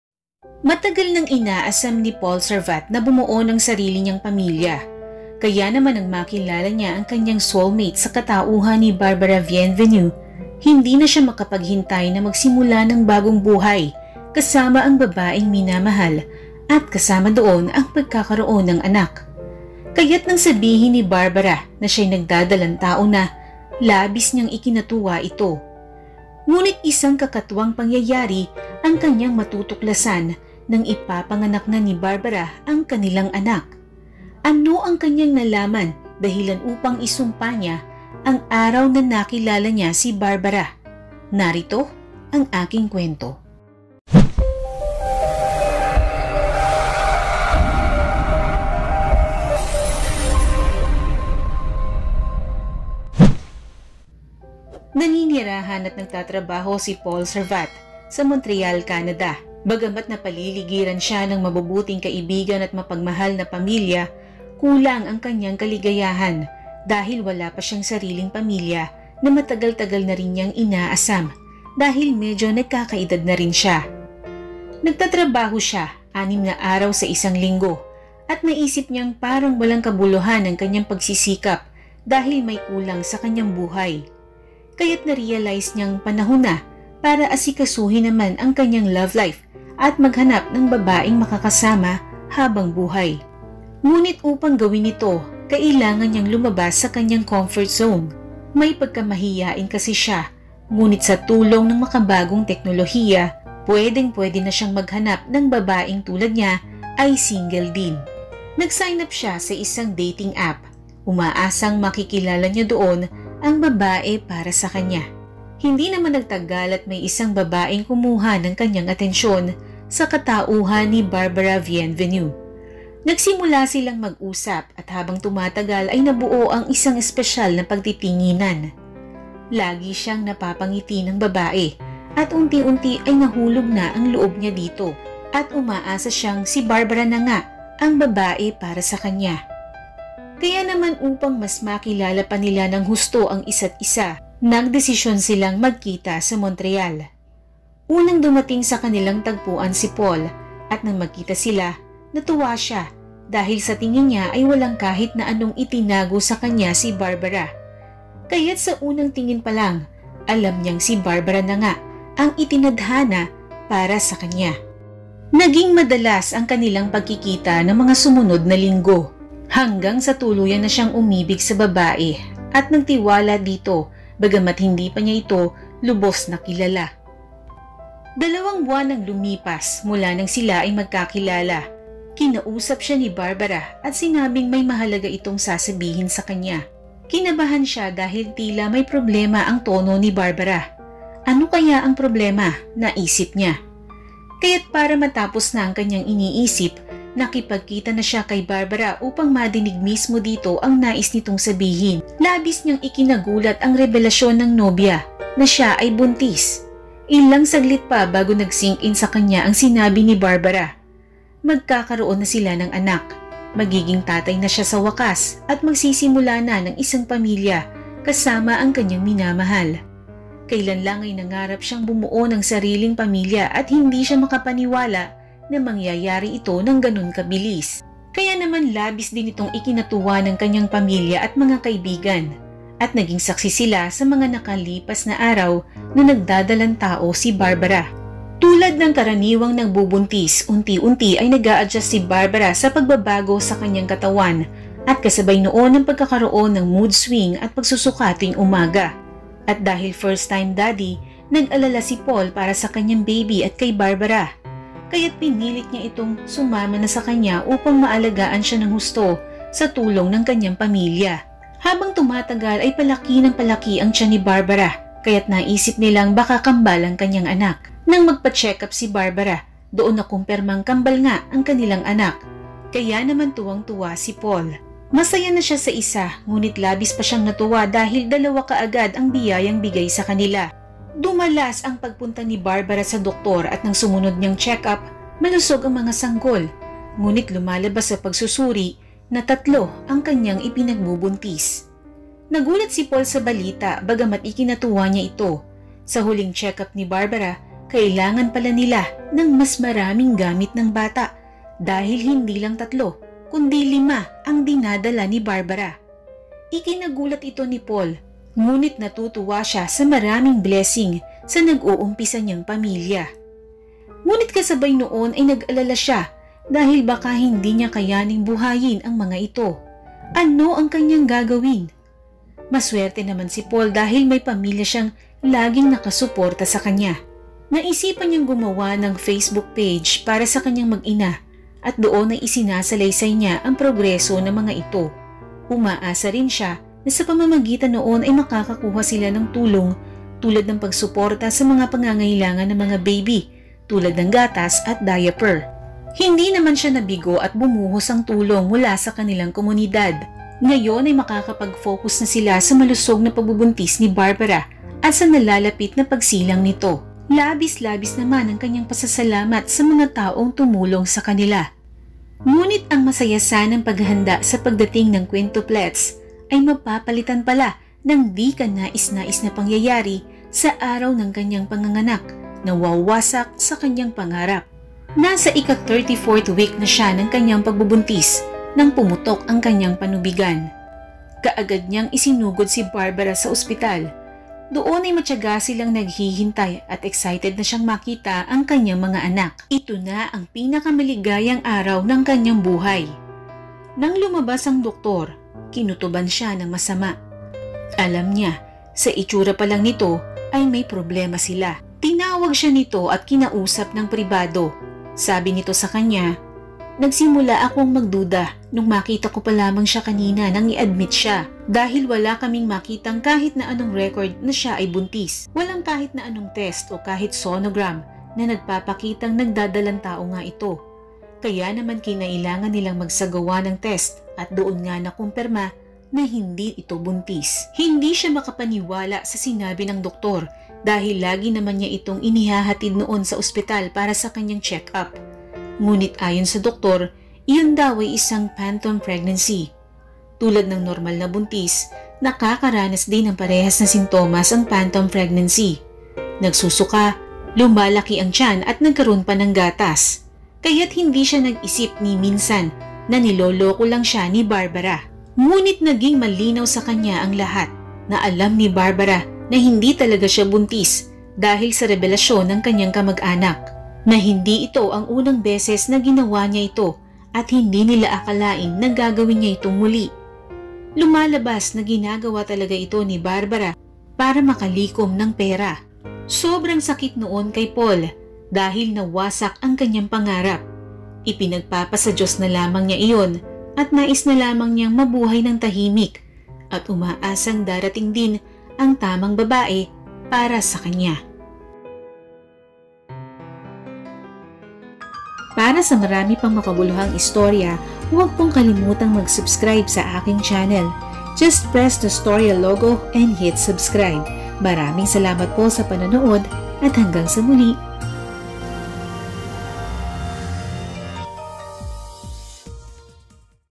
Natuklasan niya ang masaklap na katotohanan nang ipapanganak na ang quintuplets | True Story
Pangarap bumuo ni Paul Servat ng sarili niyang pamilya dahil medyo nagkakaedad na siya. Nang makilala ang isang babae sa isang dating app, naramdaman niya kaagad na siya na nga ang itinadhana para sa kanya.

Naging mabilis ang mga pangyayari at hindi nga nagtagal ay nabuntis niya ito sa hindi lang isa kundi quintuplets ang dadagdag sa binubuo niyang pamilya.

Ngunti isang pangyayari ang gigimbal sa kanya sa araw ng panganganak ng nobya na wawasak sa kanyang pangarap.